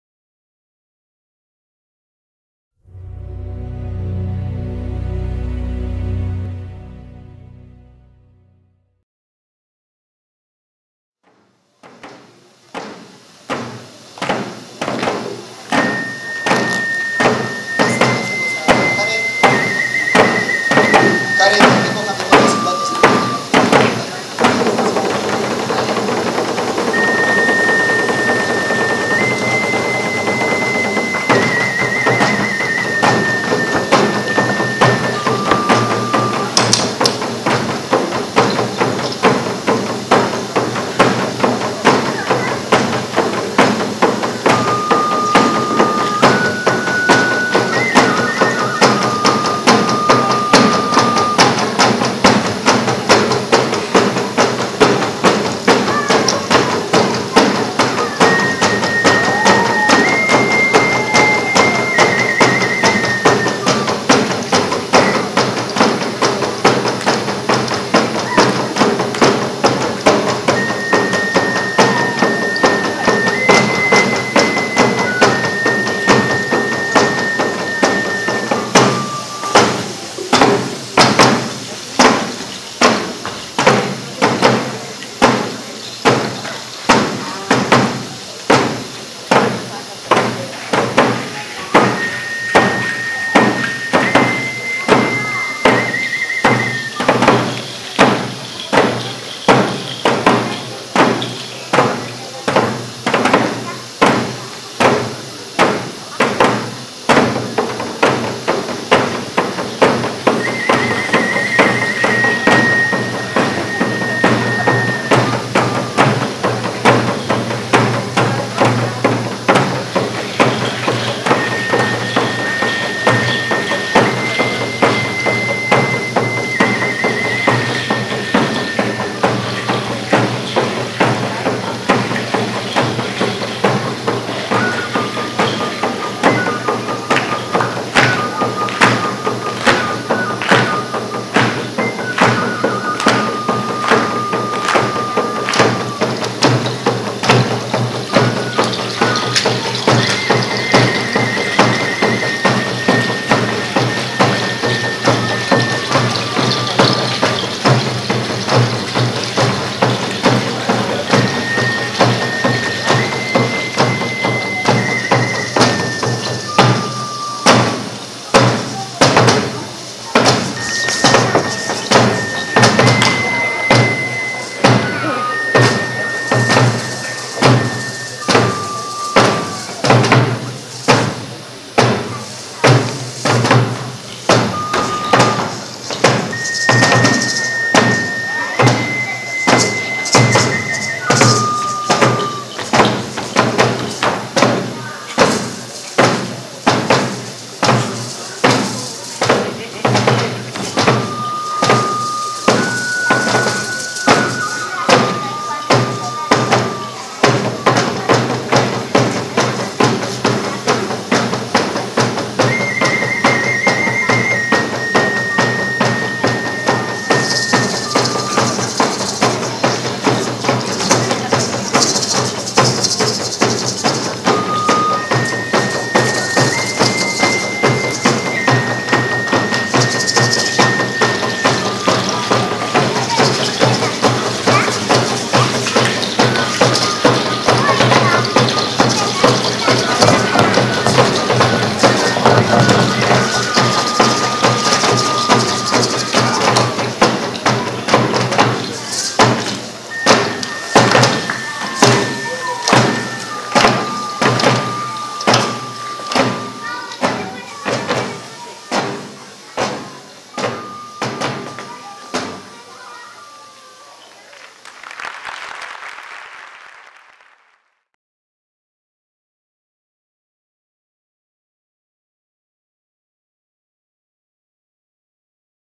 Danza de los Pascolas y el venado
LA historia de la danza en un pequeño mundo,  Universidad Infantil José Martí 2016